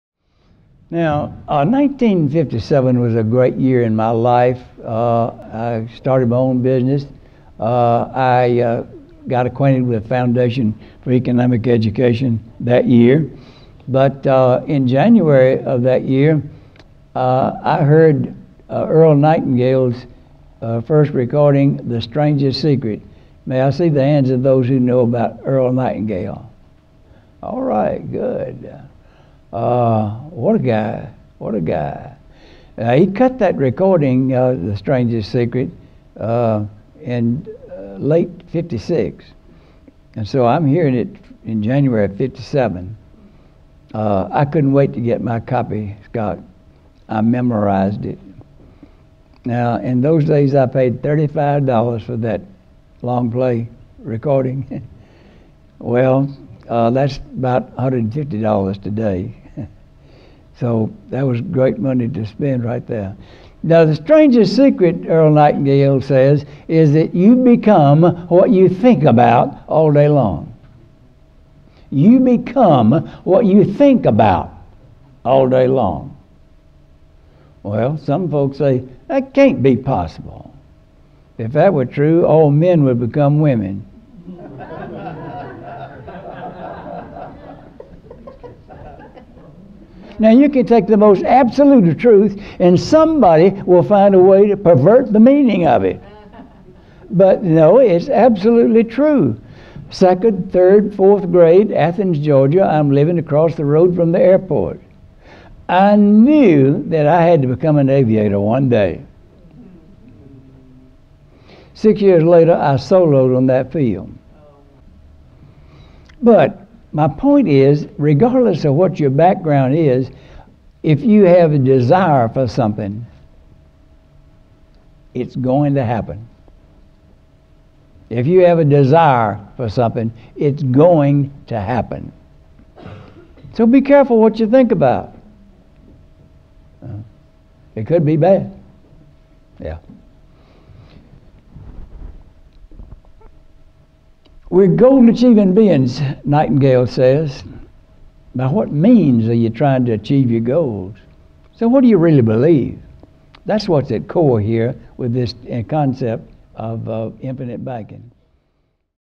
Infinite Banking & Earl Nightingale's The Strangest Secret — Nelson Nash
Best selling author and speaker R. Nelson Nash describes his first experience with Earl Nightingale and how it relates to your goals with infinite banking. Enjoy!
Become a client!
What is the Infinite Banking Concept®?
The truth about all Qualified Retirement Plans (IRA, 401(k), 403(b)